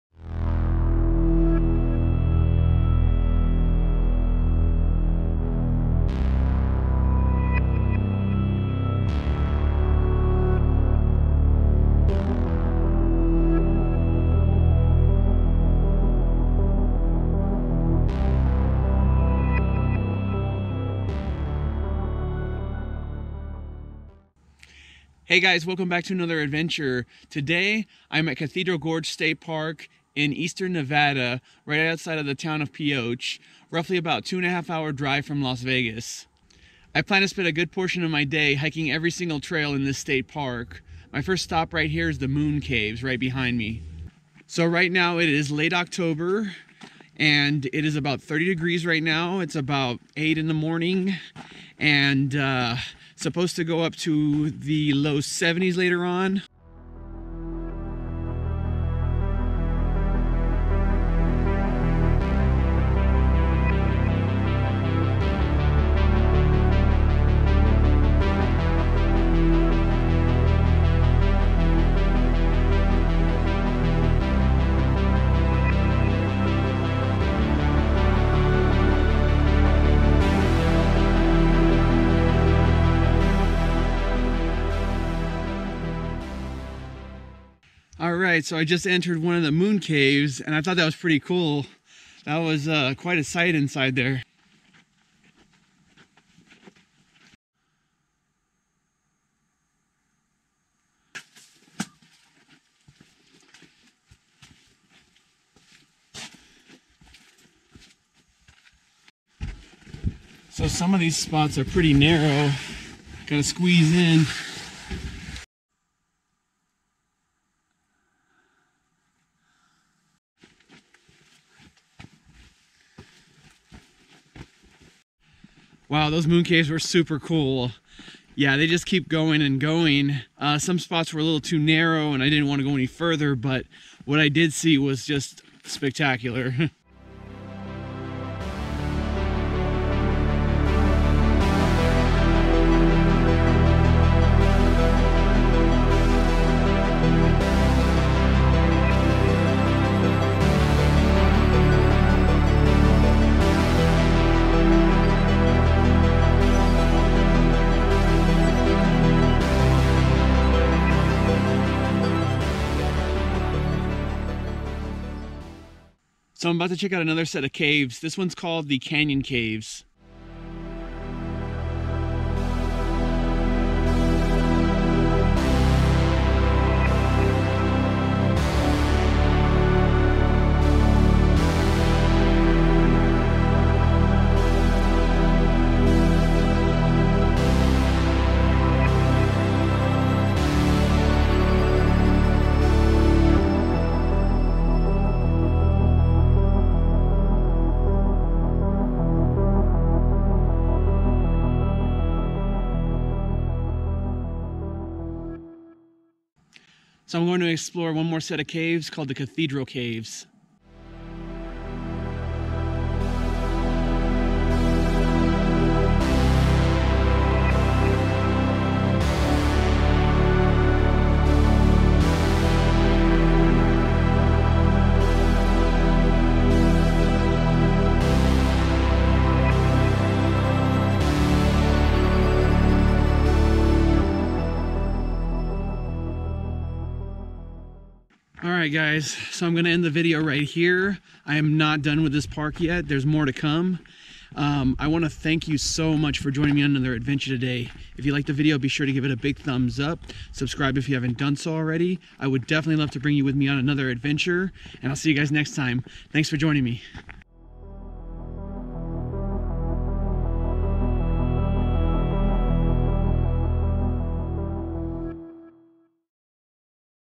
CATHEDRAL GORGE STATE PARK CAVES
Cathedral Gorge State Park is located in Eastern Nevada, about a 2.5-hour drive from Las Vegas.  This is a very unique one of a kind park that has cool rock formations and even cooler caves.  In this video, I explore the 3 major cave areas of the park. They are all in pretty close proximity to each other, and you get to see  how amazing they all are!